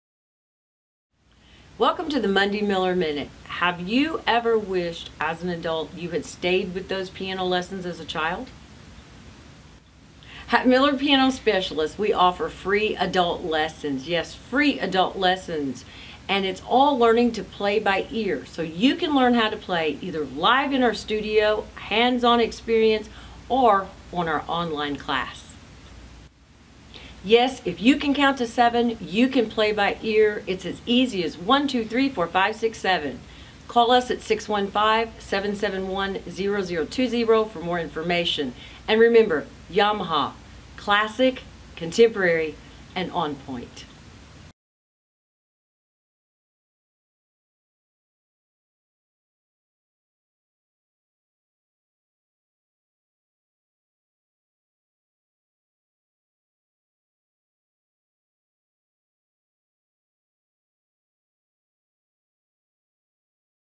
MMM Sherry adult class
Monday Miller Minute introduces adult play by ear class - free of charge!